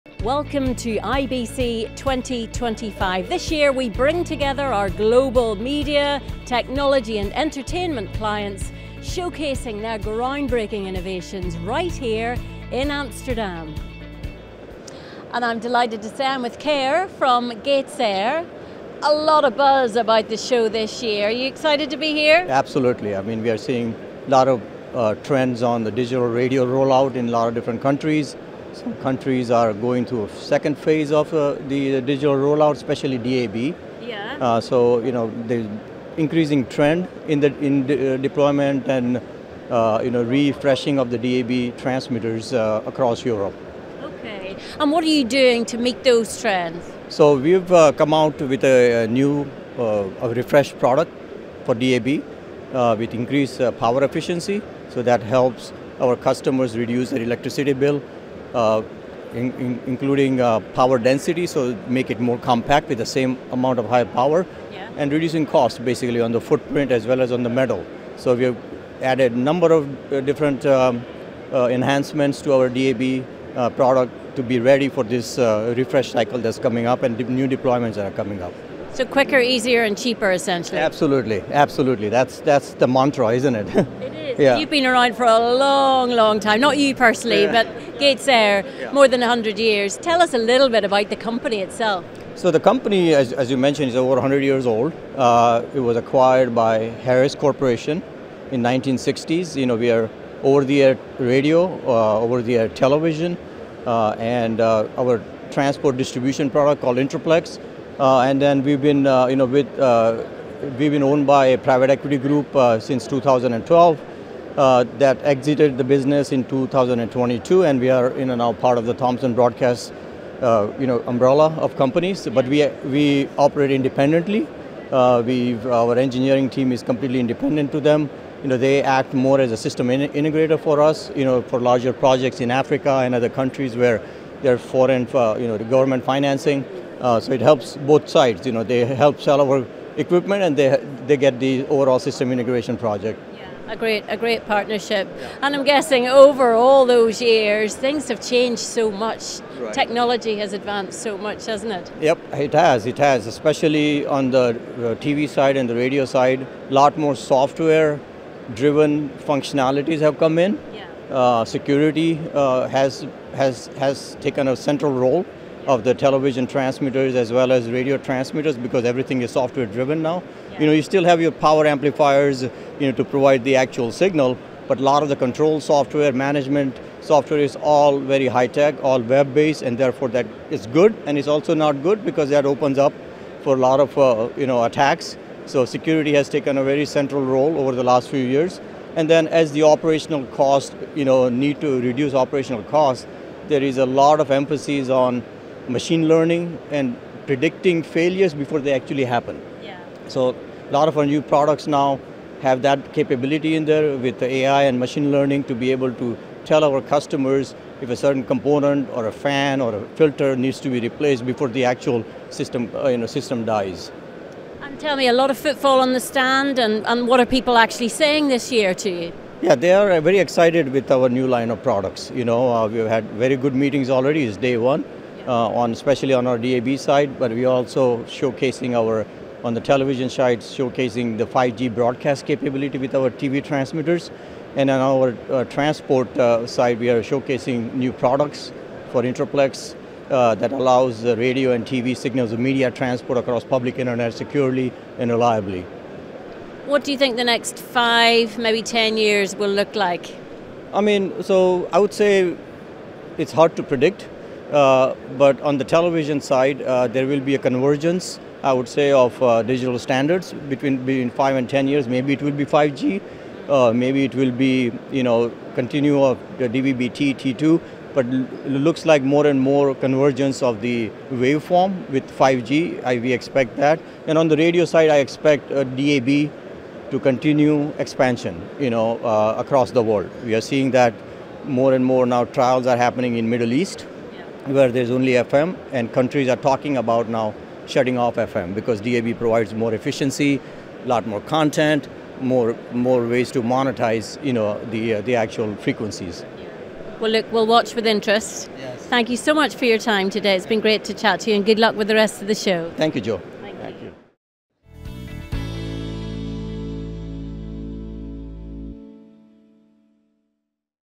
GatesAir Reveals DAB Radio Expansion Plans & 5G Broadcast Future at IBC2025
Join us at IBC2025 in Amsterdam as we chat with Keyur Parikh, VP of Engineering at GatesAir, to discuss several exciting developments in over-the-air broadcasting technologies. In this exclusive interview, we explore:

 GatesAir's enhanced power-efficient DAB transmitters with reduced operational costs
The second phase of DAB digital radio rollouts across Europe
How AI and machine learning are revolutionizing broadcast equipment maintenance
5G broadcast capabilities for television transmitters
Future convergence of digital standards and global DAB expansion

Discover how this century-old broadcast technology leader is adapting to modern challenges with smarter, more efficient solutions that help broadcasters reduce electricity bills while maintaining superior performance.